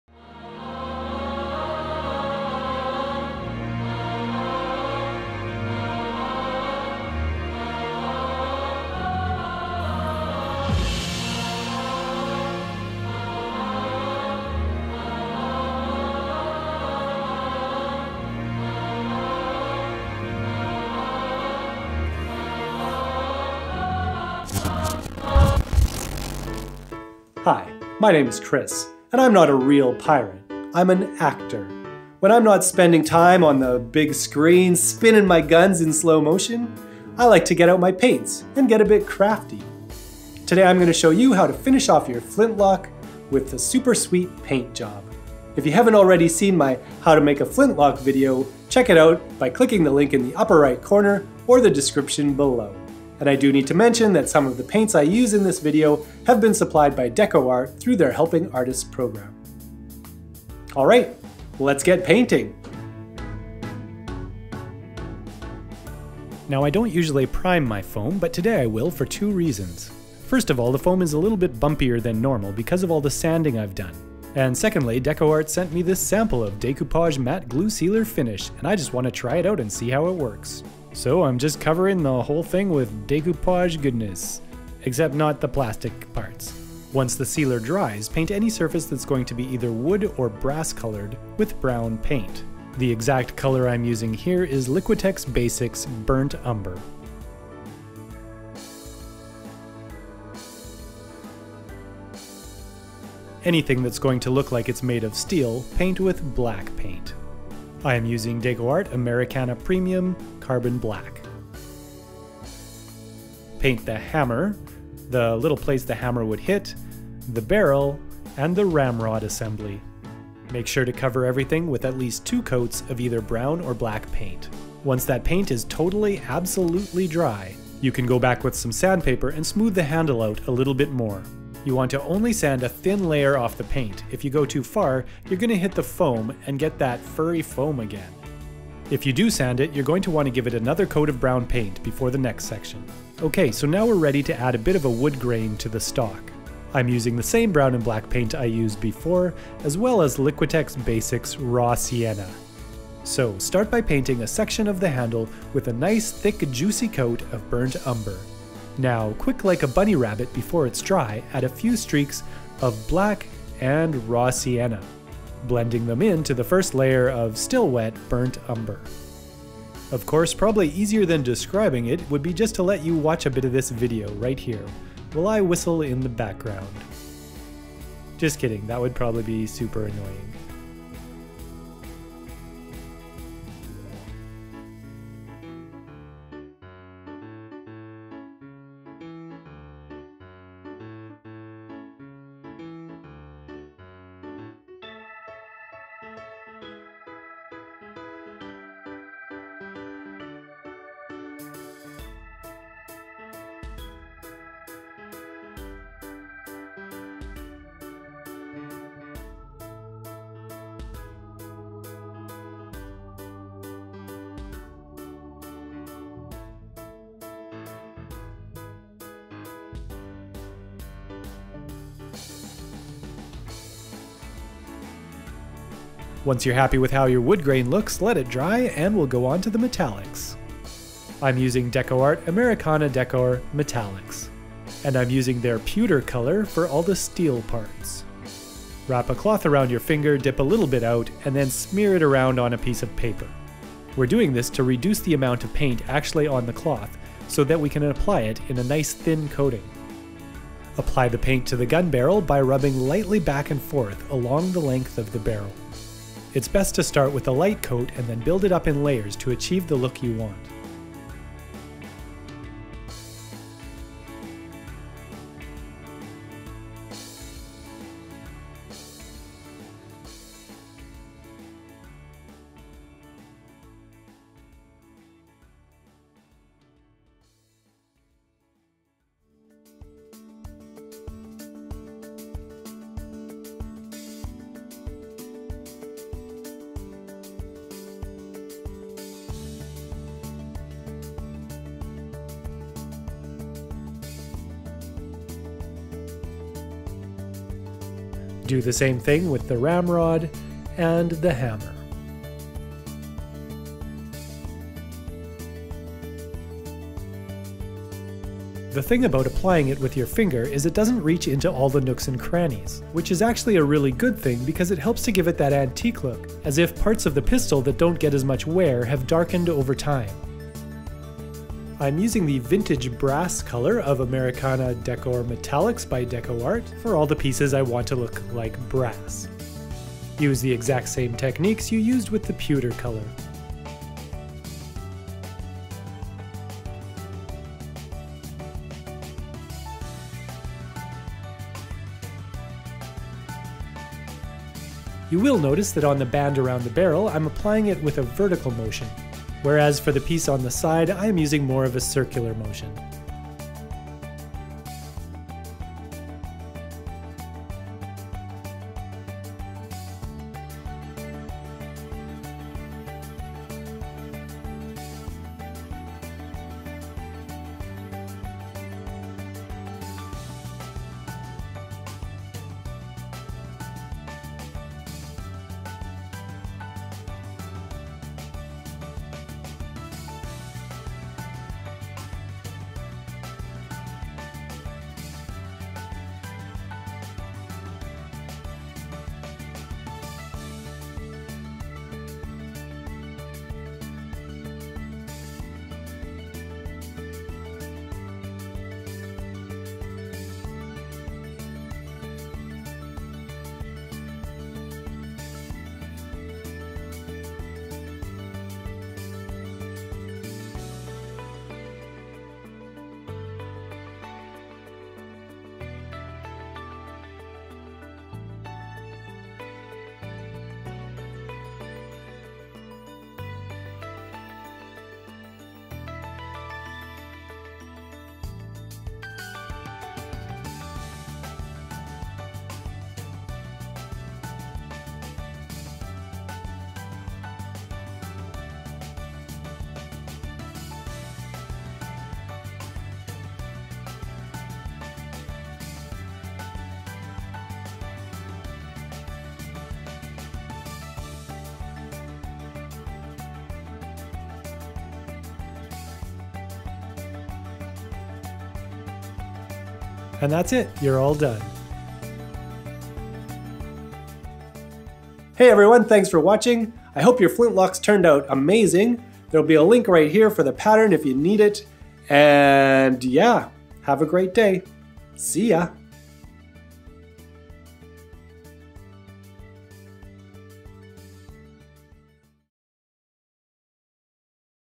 How to Make A Pirate Gun From Foam. Part 2- Prop Painting Techniques
This is the second video in the foam flintlock tutorial. If you missed the first one, check it out here➜

In this video I show how I painted the flintlock pistol, giving the stock a woodgrain finish, and applying the metallic paints to the brass and steel parts. I take a little more time than usual so that you can see the painting process in real time rather than having quick cuts, hopefully to give a better feel for the painting technique.

Full disclosure: DecoArt provides me with paints to use in my videos through their Helping Artists program.